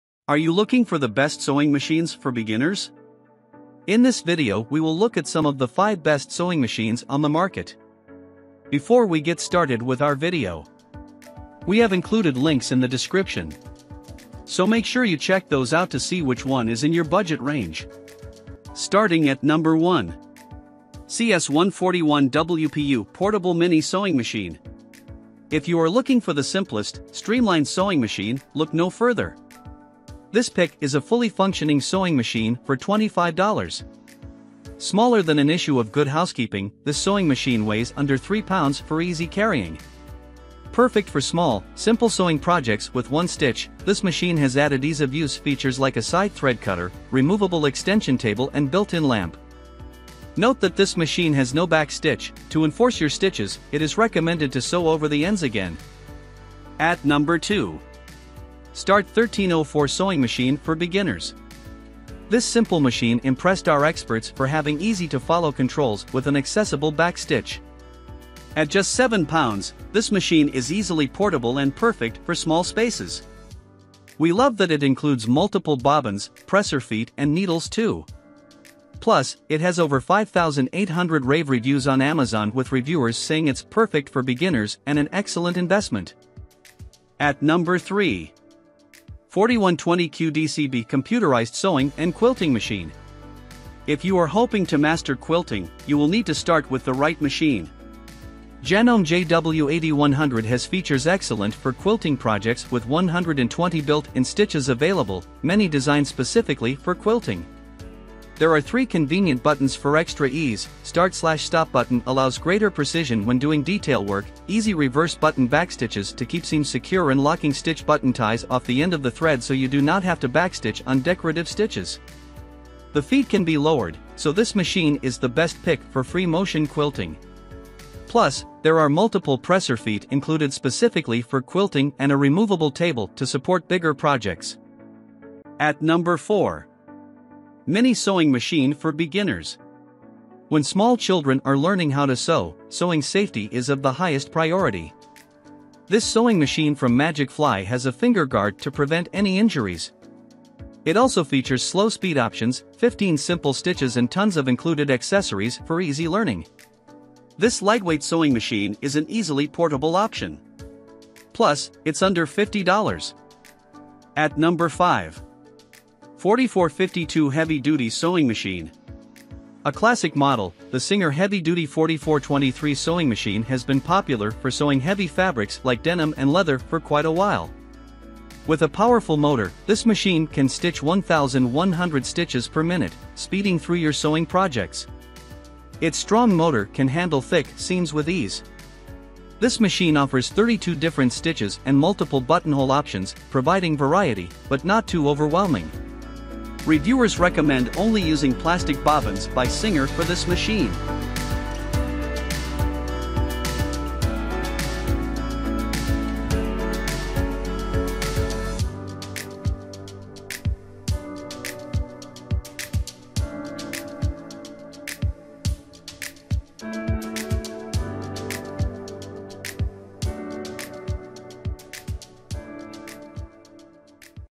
✅Best Sewing Machines for Beginners in 2022 | Top 5 Best mini sewing machine Reviews in 2022
📌Product Link📌:

In this video, we listed the Best Sewing Machines for Beginners